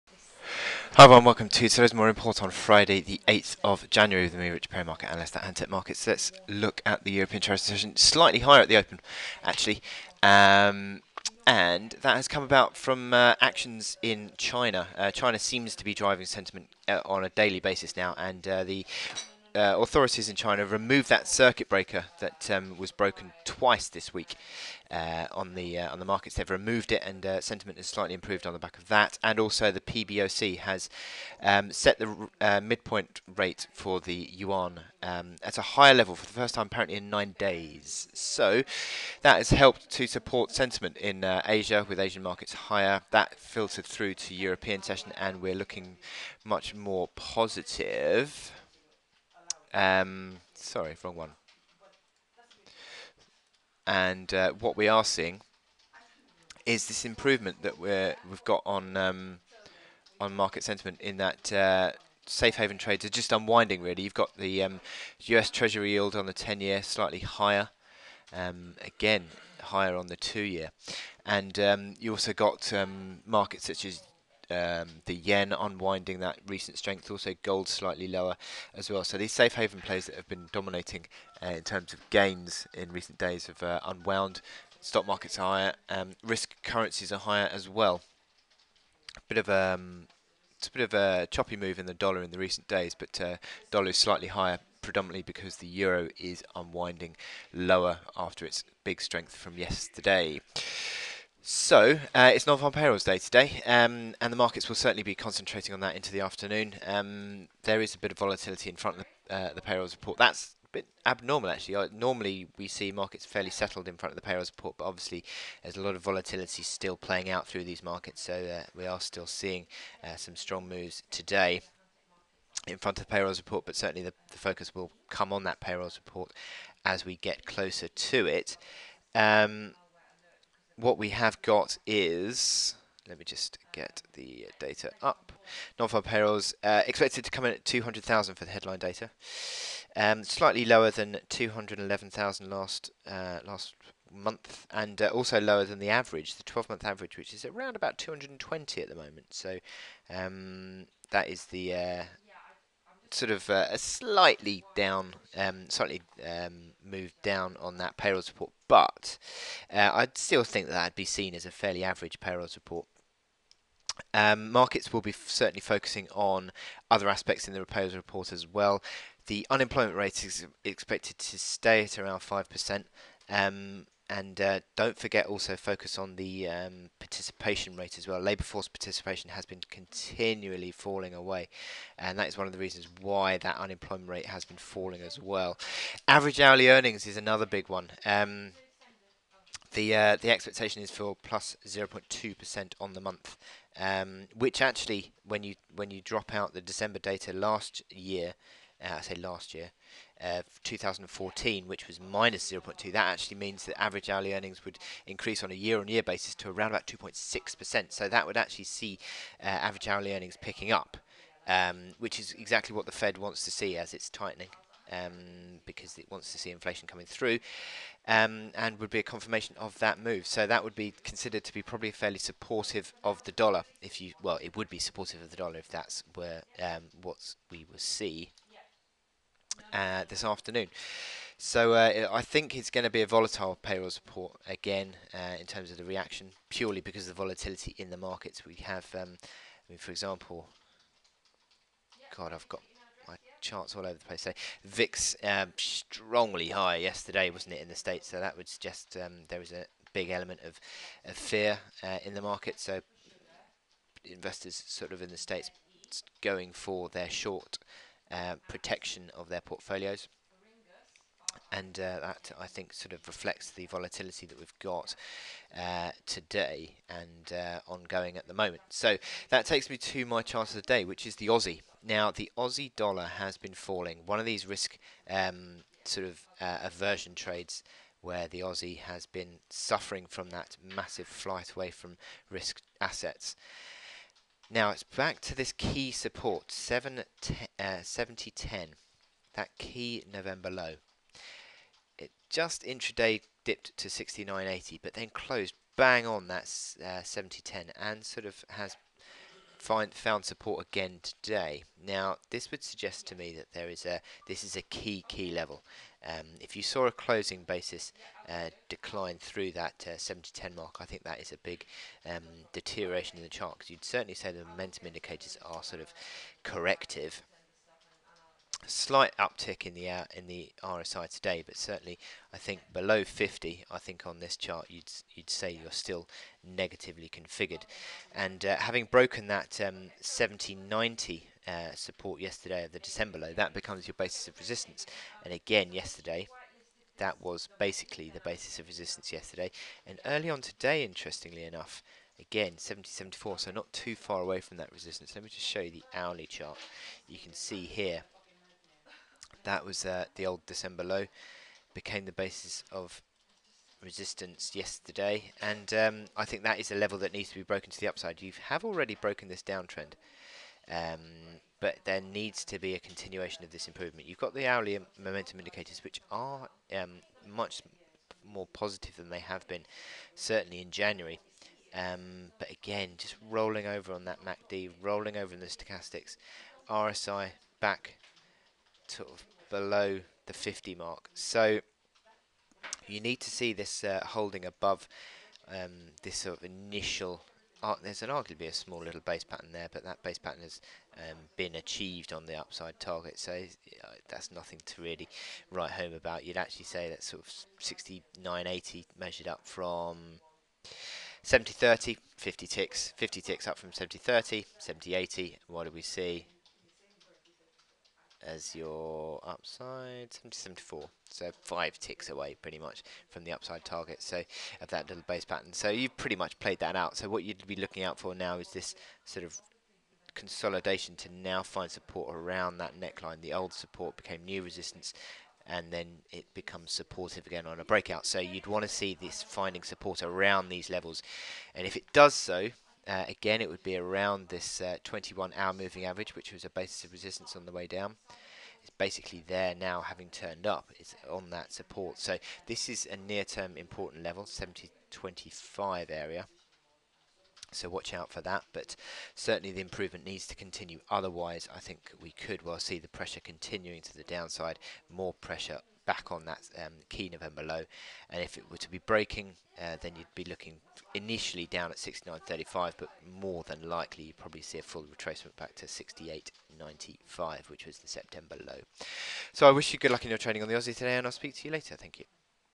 Daily Market Overview 08/01/2016
What are the key drivers as risk appetie has taken a boost. Richard also looks forward to Non-farm Payrolls. The chart of the day is Aussie dollar